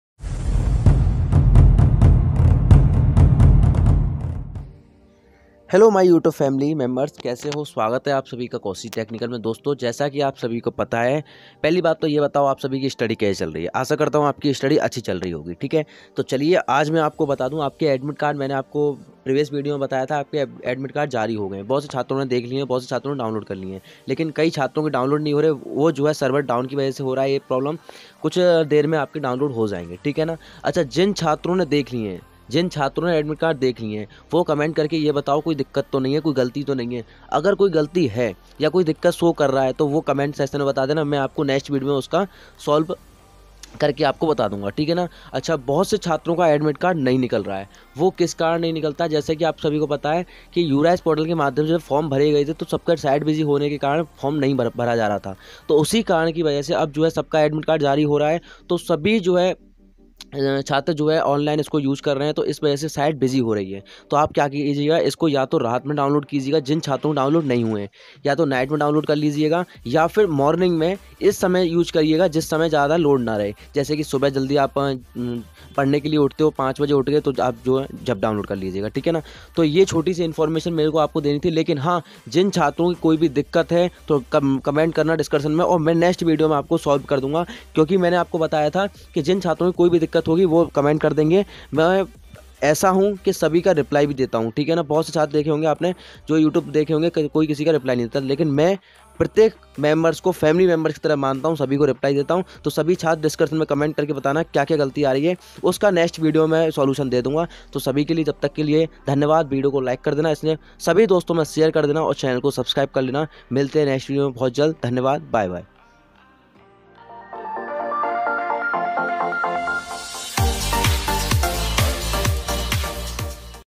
Polytechnic Admit Card बैक पेपर नहीं दिखा रहा.🤔|Admit Card देखा या नहीं | क्या क्या गलती है|Bhavesh
Polytechnic Admit Card बैक पेपर नहीं दिखा रहा.🤔|Admit Card देखा या नहीं | क्या क्या गलती है|By-Bhavesh Kaushik

Polytechnic odd semester admit card, odd semester admit card Dekha ya nahin, admit card mein kya kya Galti Hai, yah Galti Kaise Sahi Karen

 पॉलिटेक्निक विषम सेमेस्टर प्रवेश पत्र जारी, प्रवेश पत्र में क्या क्या गलती है, पॉलिटेक्निक प्रवेश पत्र में बैक पेपर  डिटेल नहीं दिखा रहा

 पॉलिटेक्निक विषम सेमेस्टर प्रवेश पत्र जारी, जल्दी करें डाउनलोड, जिन छात्रों का प्रवेश पत्र में कोई भी प्रकार की त्रुटि है, वह जल्दी देखें,

More Latest Update.